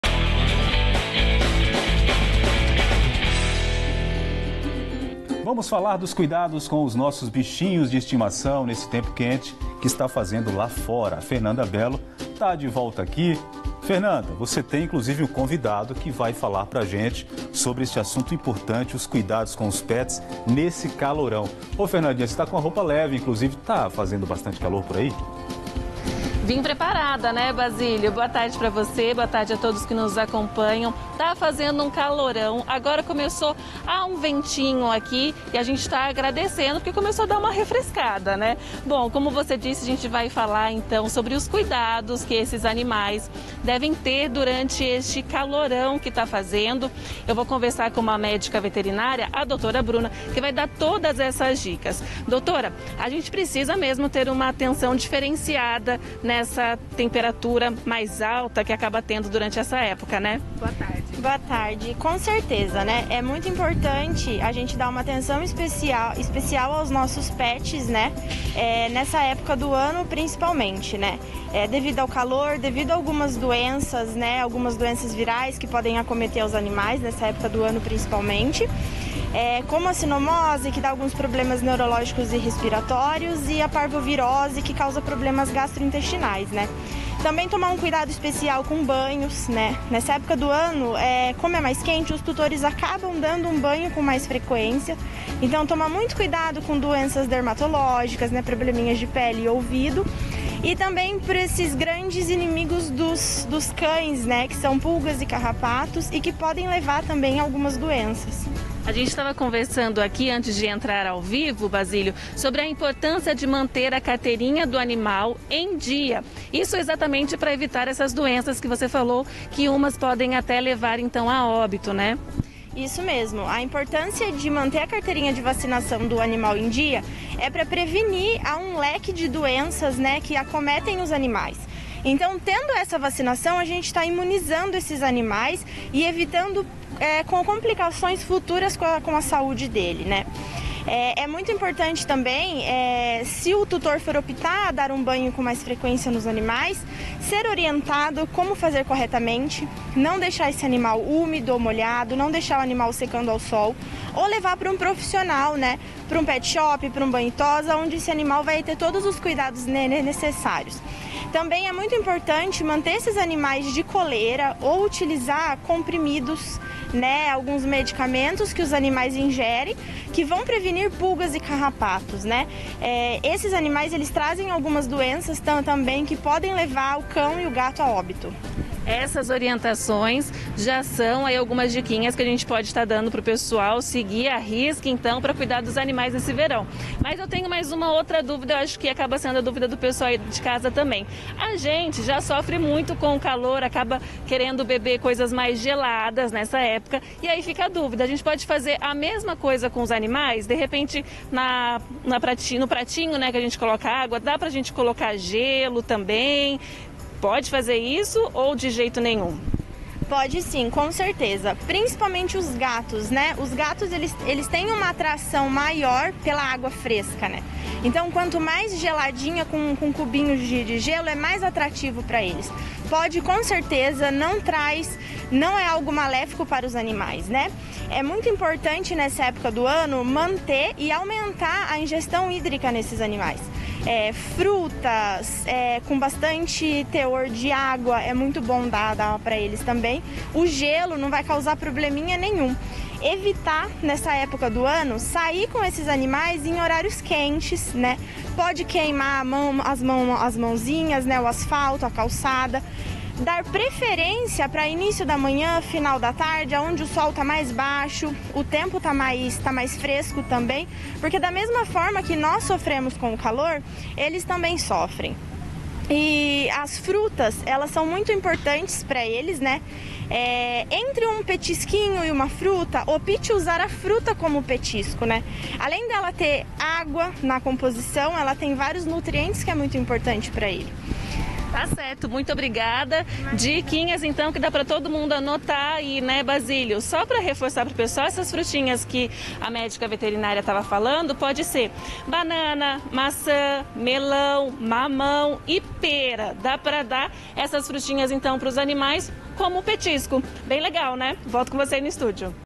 Pets precisam de cuidados especiais no verão | TV Sorocaba SBT
Nessa época do ano, não é só a gente que precisa ter cuidado nesse calorão. Os pets também sofrem com a onda de calor e precisam de cuidados especiais.